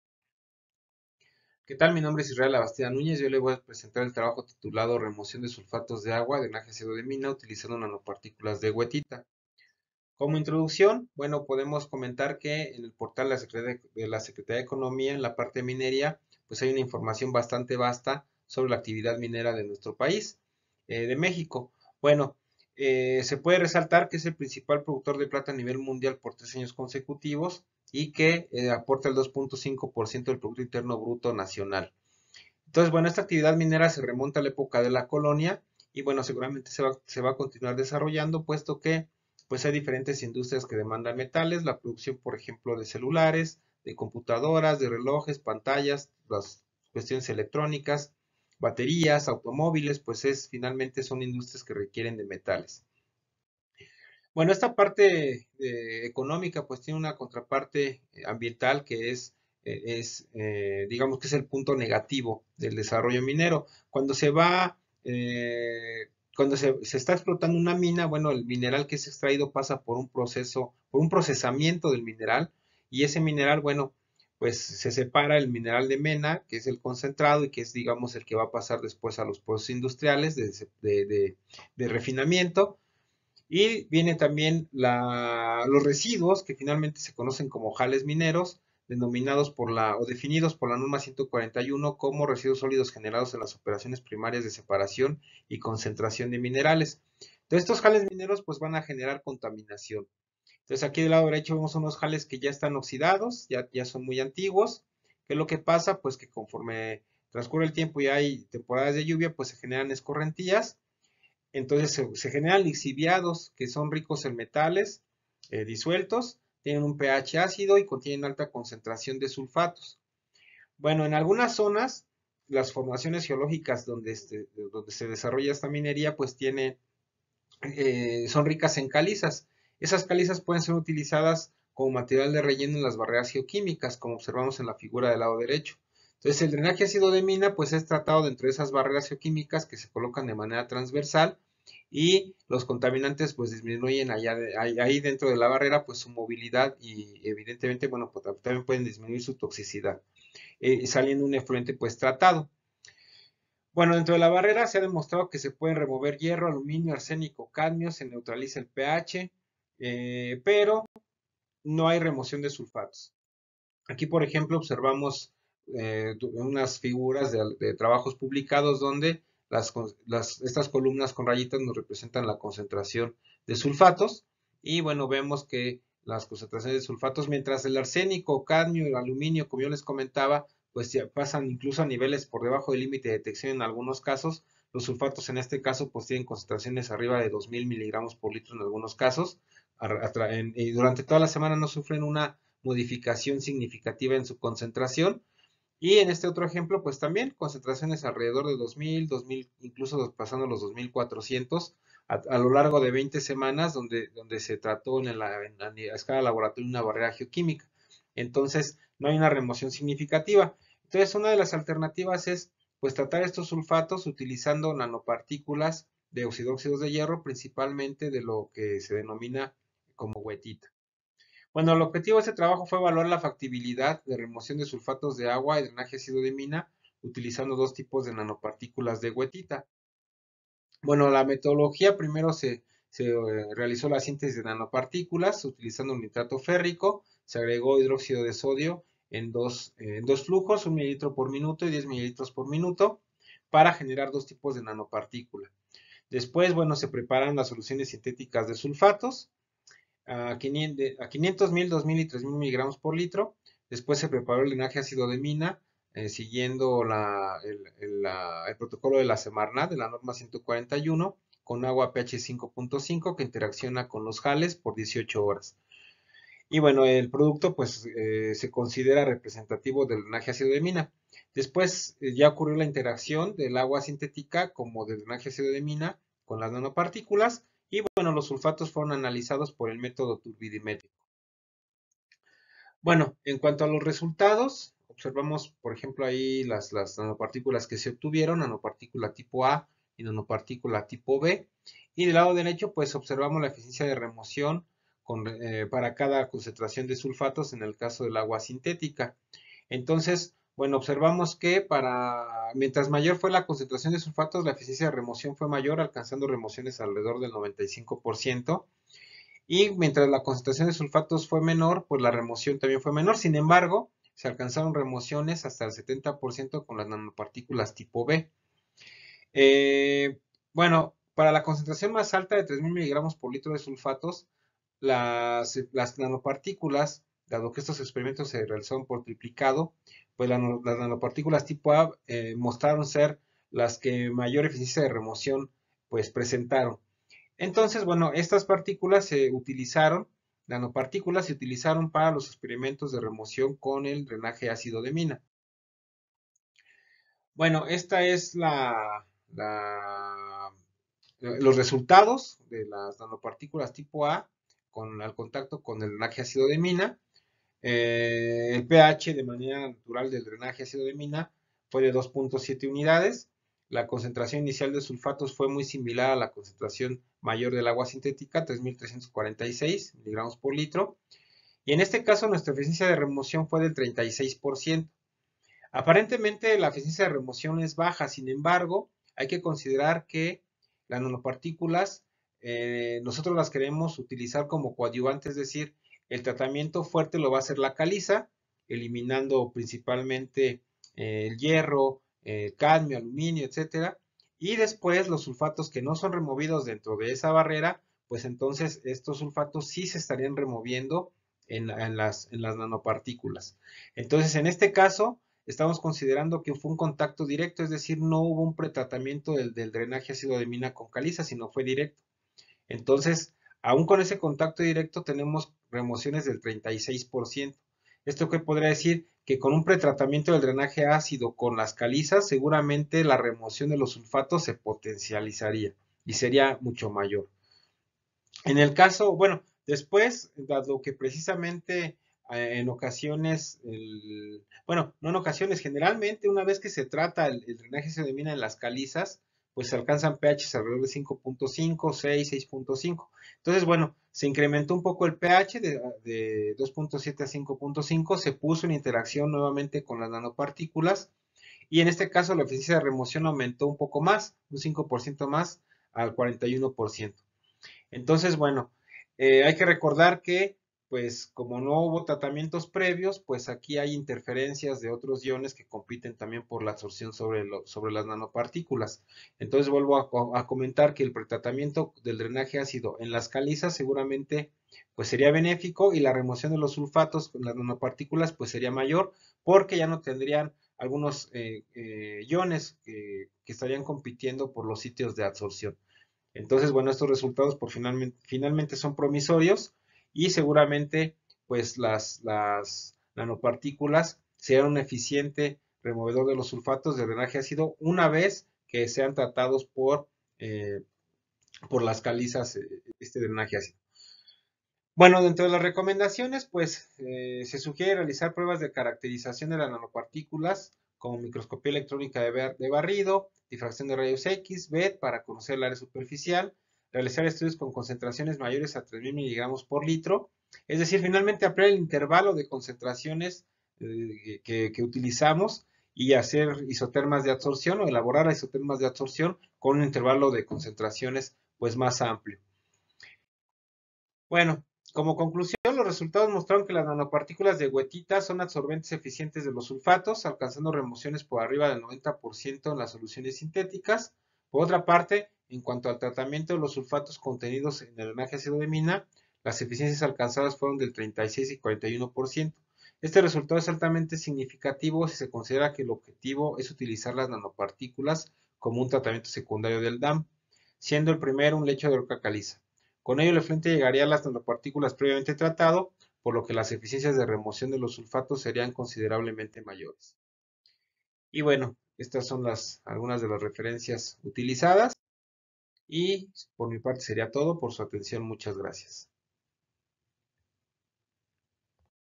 SAB2022 107 Labastida:Remoción de sulfatos de agua y drenaje ácido de mina utilizando nanopartículas
Remoción de sulfatos de agua y drenaje ácido de mina utilizando nanopartículas de goethita